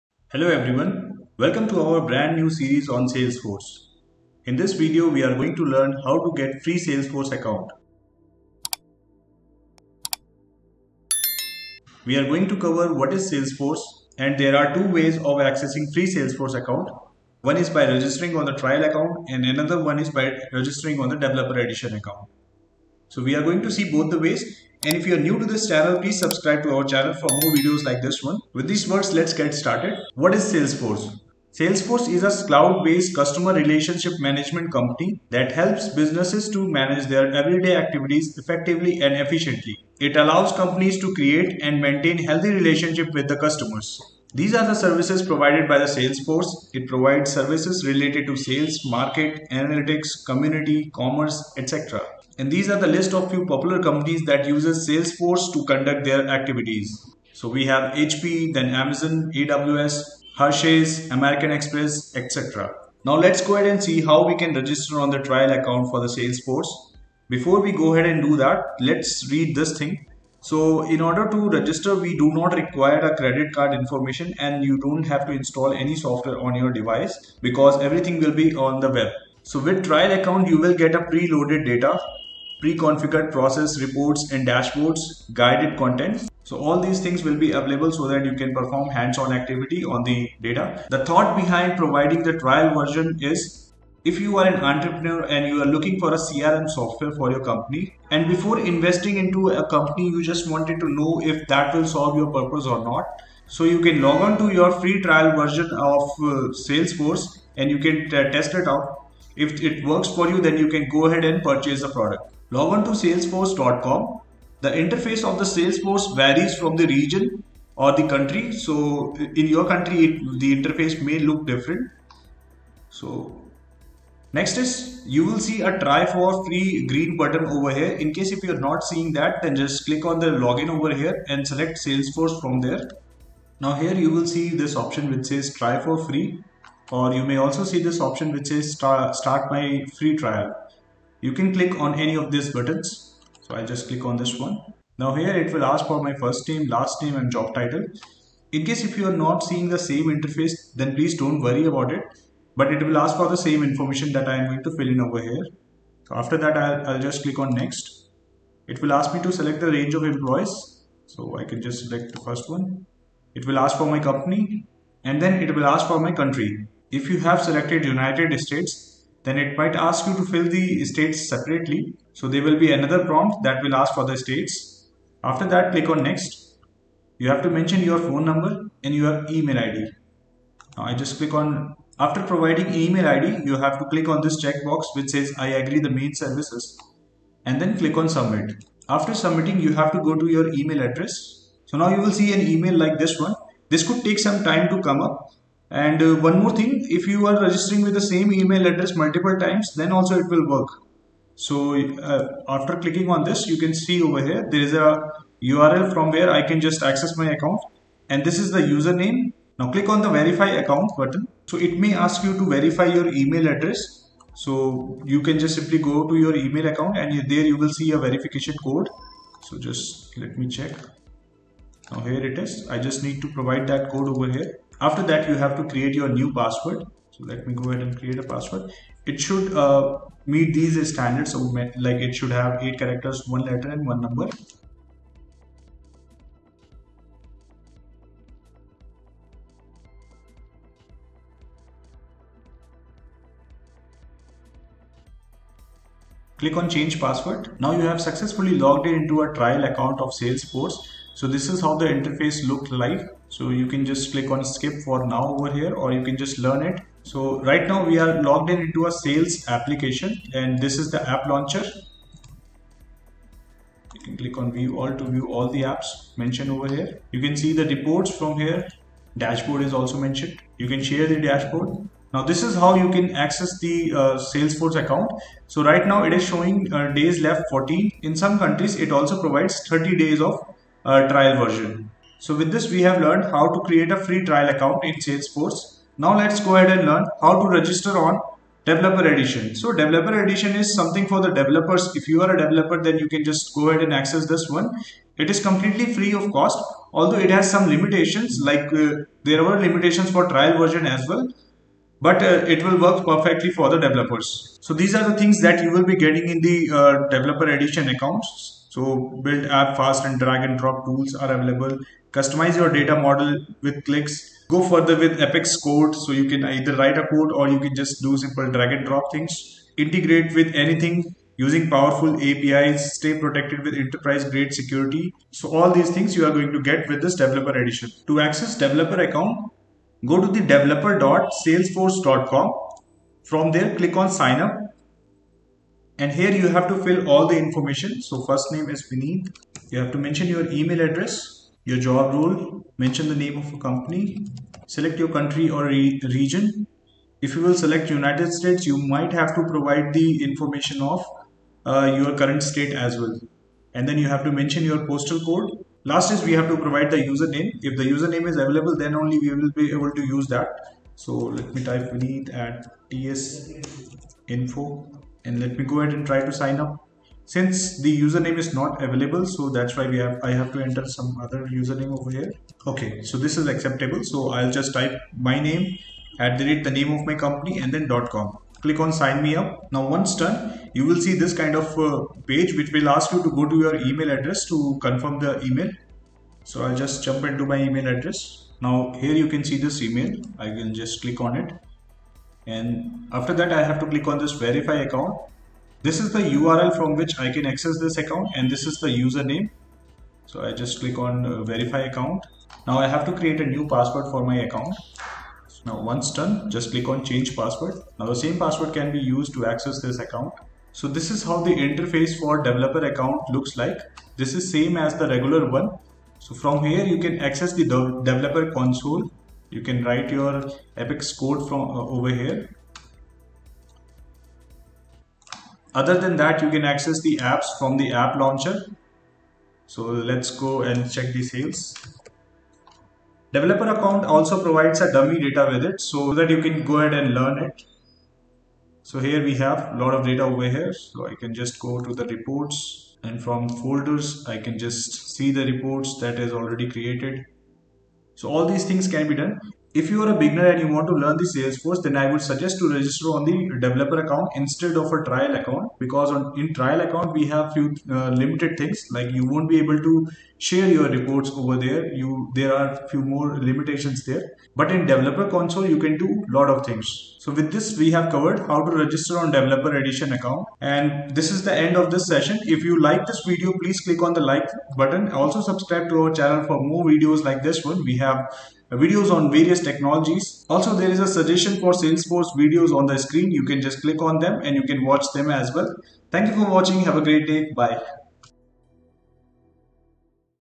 How to Create Free Account in Salesforce
In this salesforce video tutorial, I have explained How to get a free account in salesforce.
Additionally, we have covered different topics in Salesforce for a particular tasks using examples.
What is Salesforce
How to register on Trial Account in Salesforce
How to register on Developer Edition in Salesforce
Time Stamps:
0:29- What is Salesforce
1:03- Register on trial account in Salesforce
4:47- Register on developer edition in Salesforce
You can check out a complete tutorial on How to get a free salesforce account
Want to learn machine learning? Check out a complete training course on Python and Machine Learning Training Course